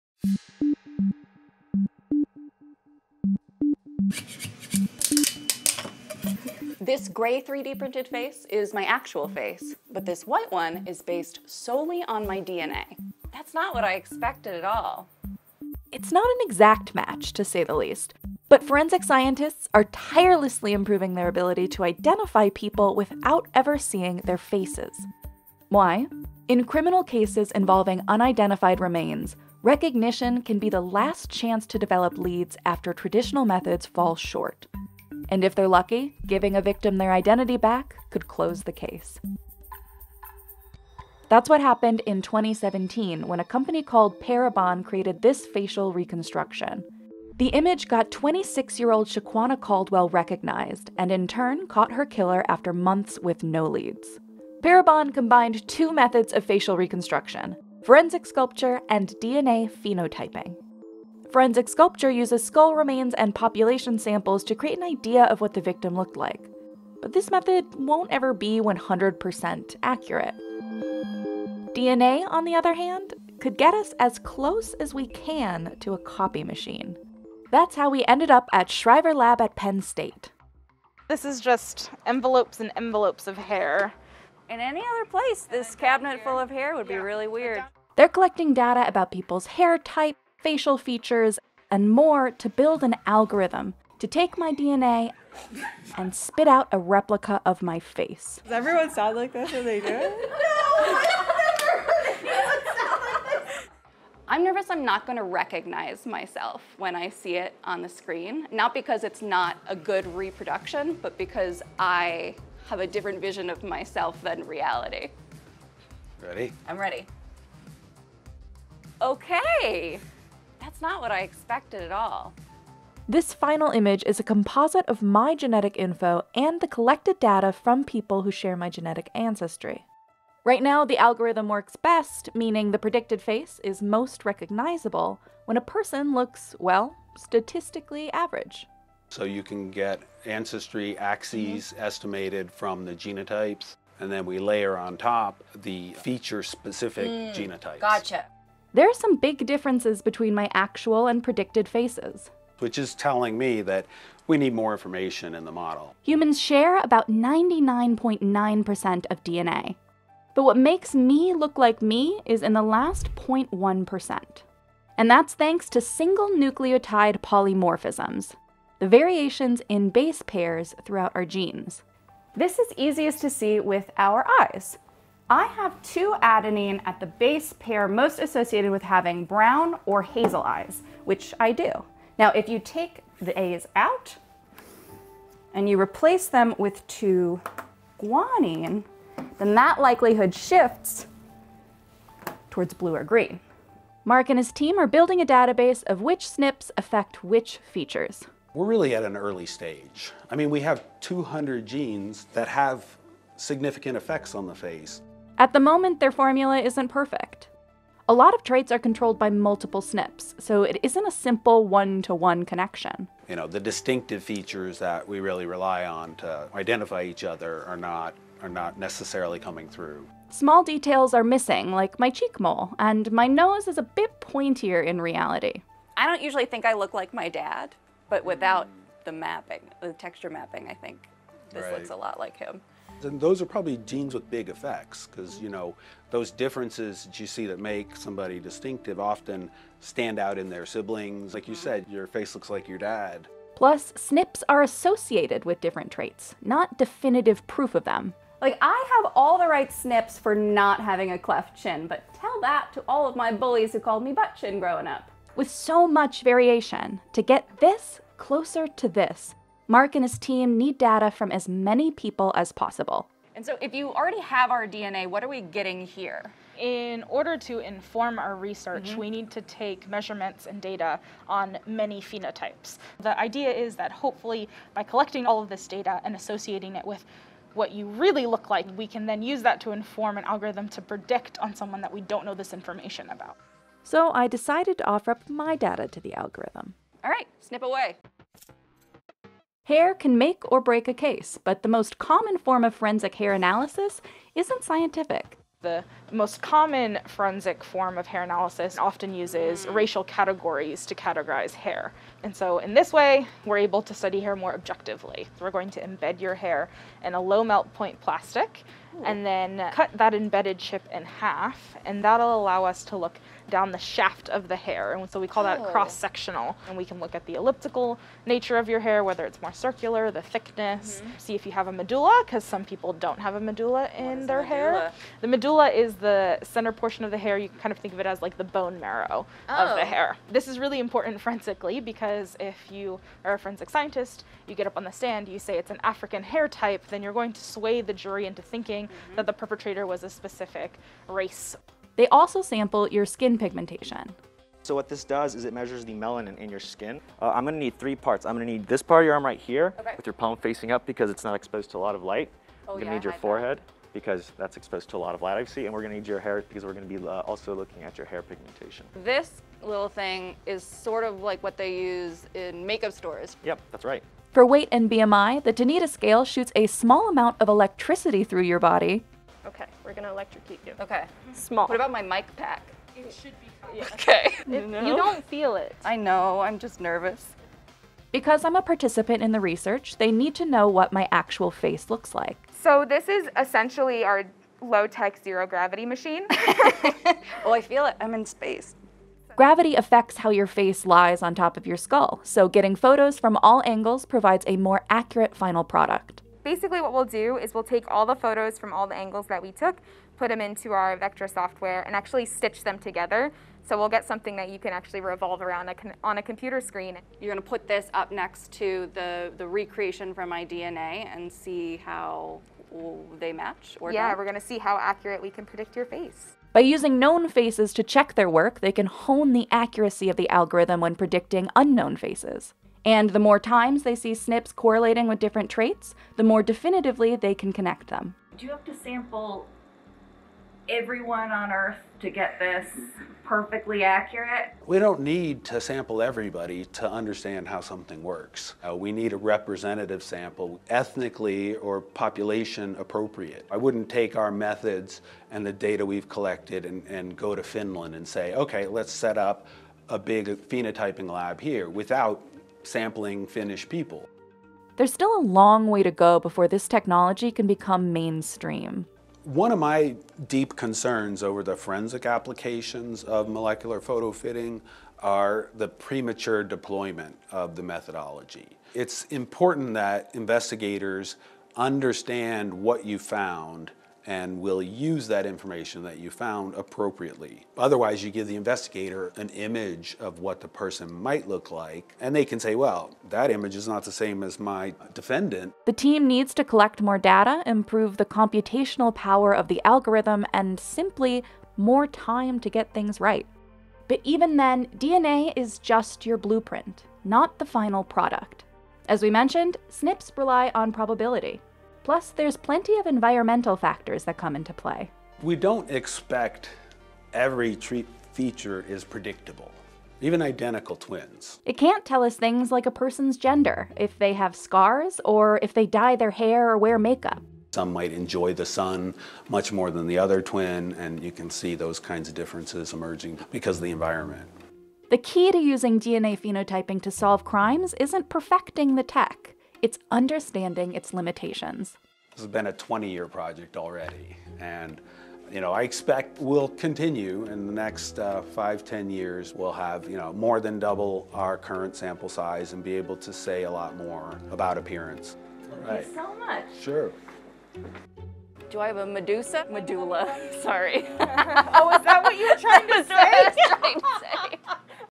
How Far Scientists Go To Create Your Face From Your DNA | Science Skills
Population geneticist Mark Shriver and his team at Penn State want to make replicas of faces using solely DNA. This kind of technology, once honed, could be essential for solving cold cases involving unidentified remains. Facial reconstructions are often a last-ditch effort to identify victims when other leads don't pan out, but traditional forensic sculptures are only approximations. Here's how Shriver Lab is trying to get us as close as we can to a copy machine.